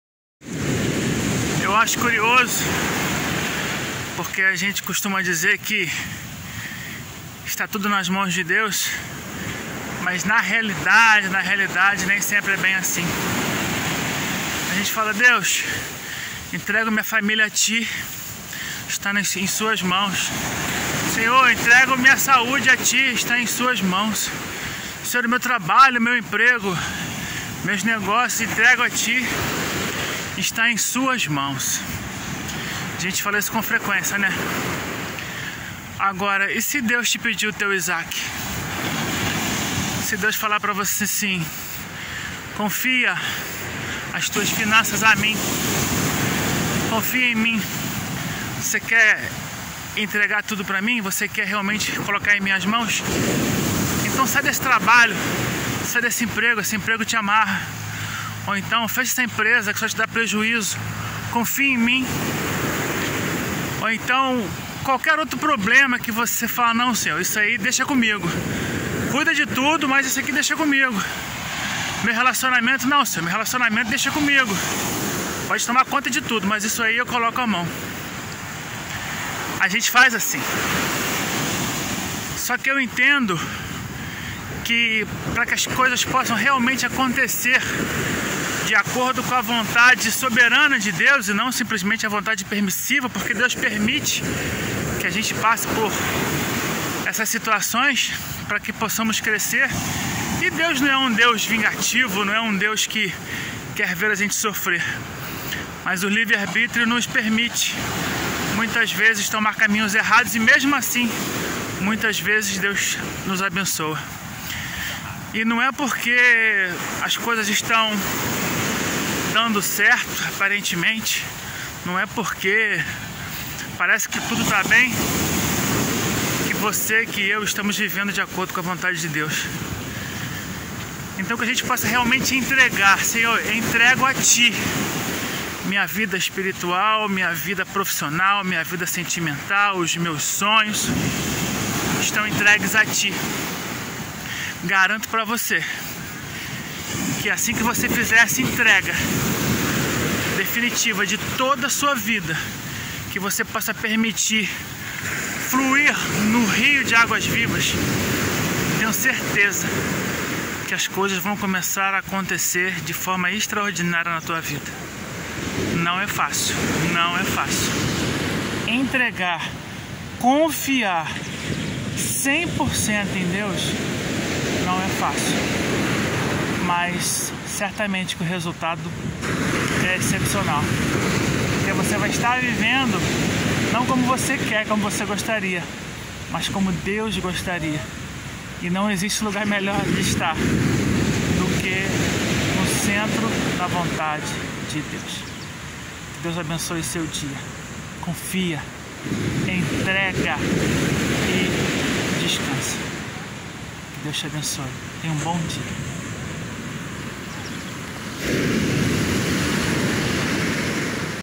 Como saber o que Deus quer? 🤔
As vezes temos o falso entendimento de dizer que "tudo" está nas mãos de Deus.

Mas a verdade não é bemmmmm assim

Compartilharei minhas experiências com você no instagram